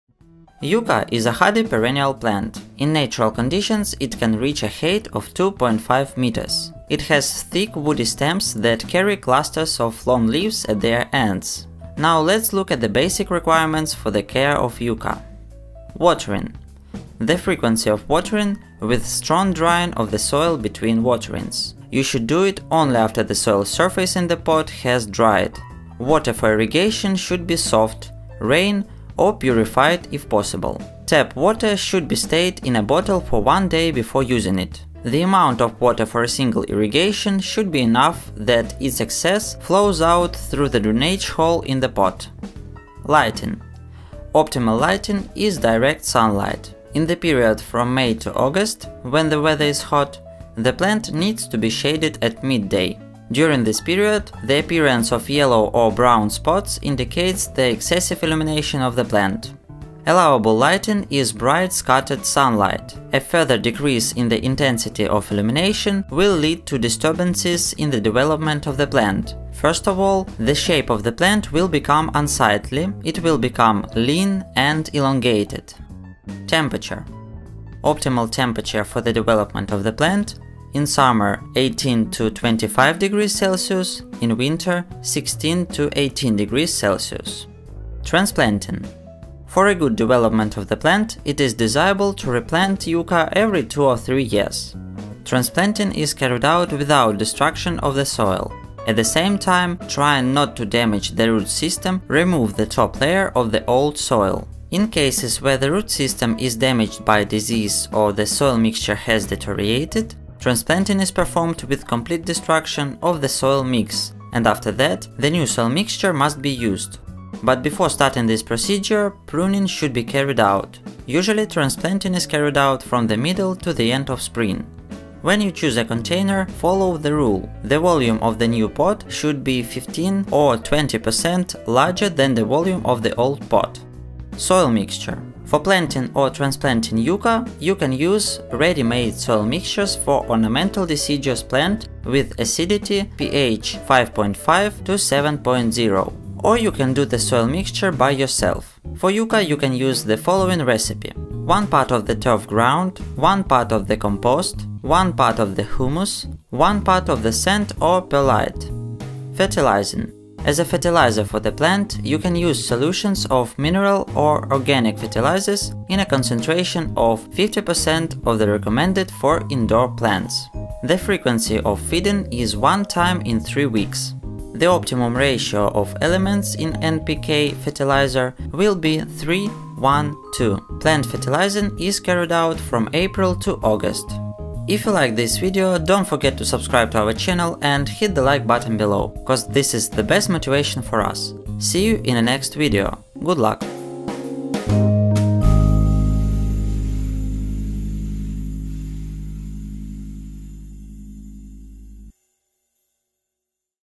YUCCA CARE CONDITIONS | Lighting, temperature, watering, soil mix
In this video we will show You most common care conditions for Yucca: lighting, temperature, watering, soil mix, fertilizing etc.